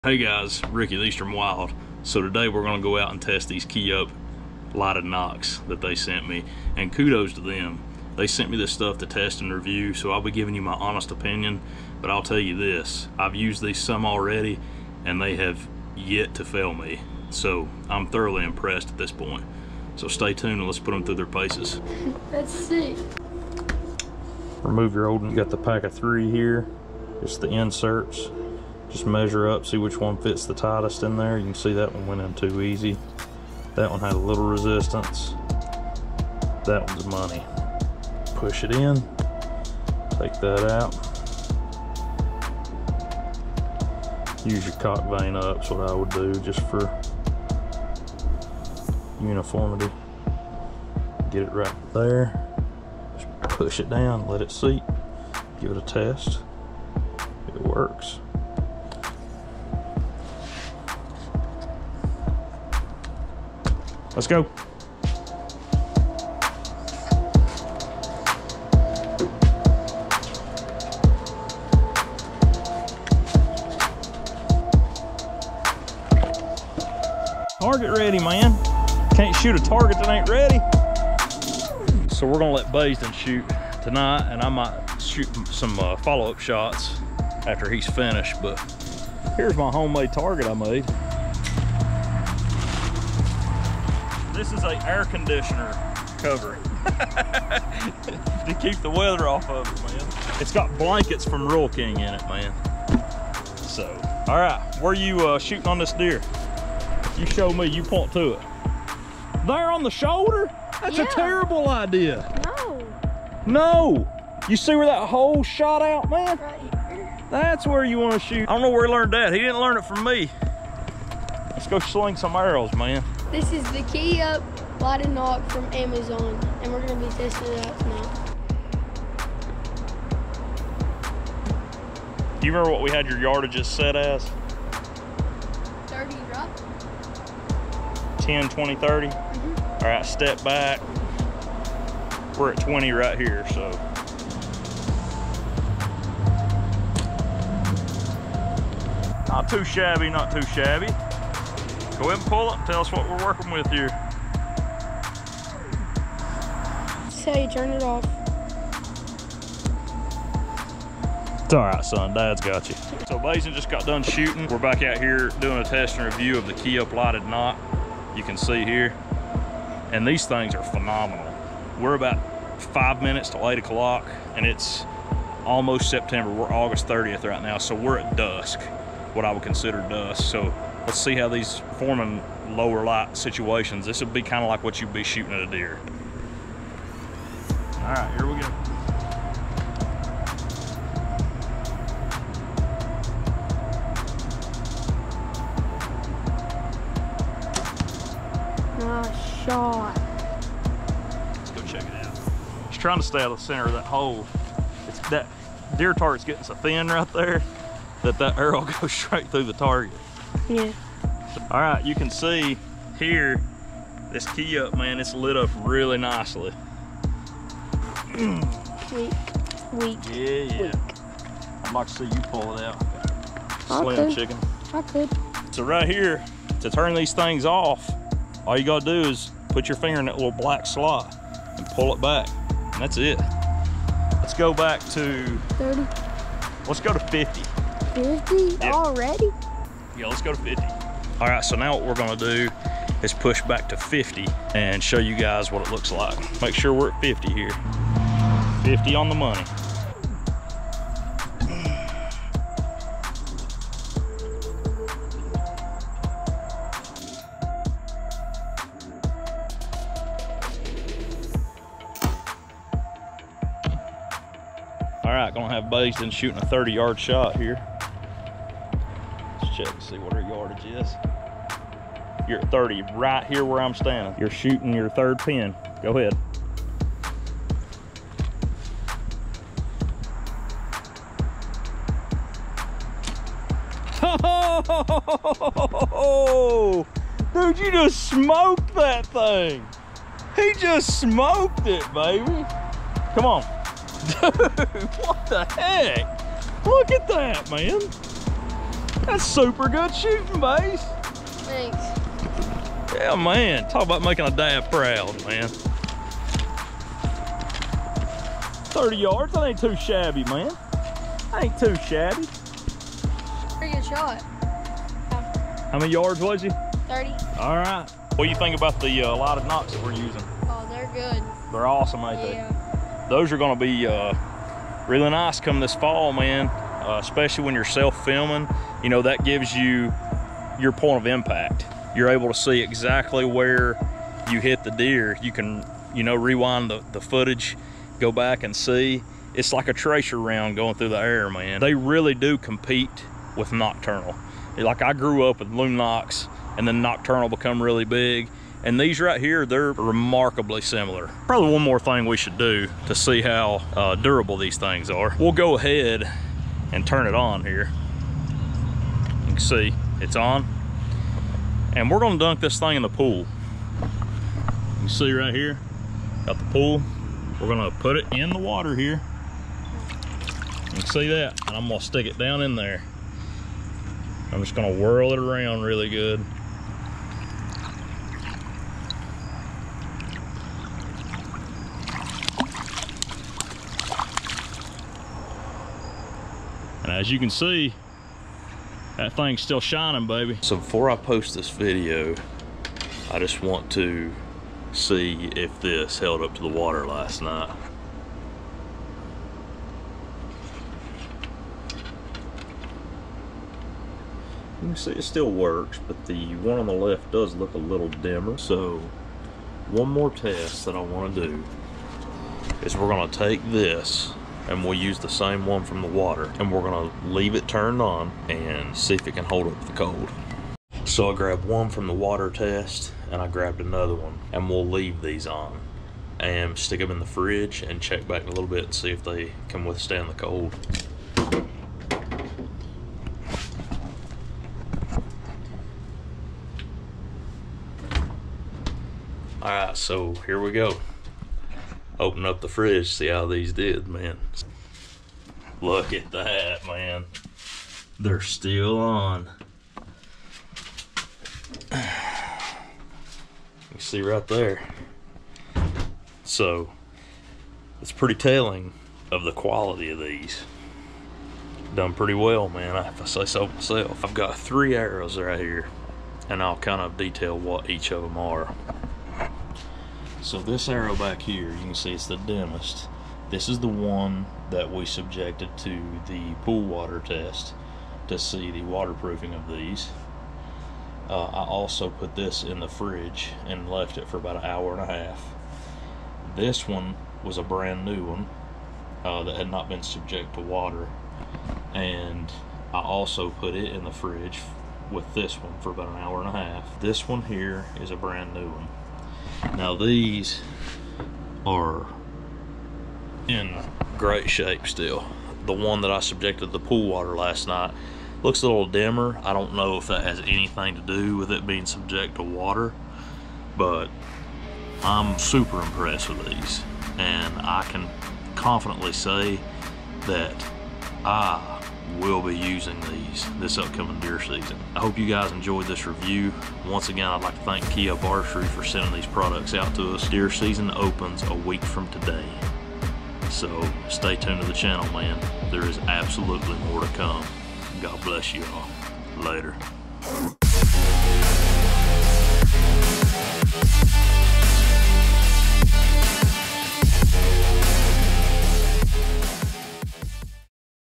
Lighted Nock Review - KEAUP Archery - Nockturnal Killers?
These things are awesome and will compete with any top tier nocks on the market! Super impressed!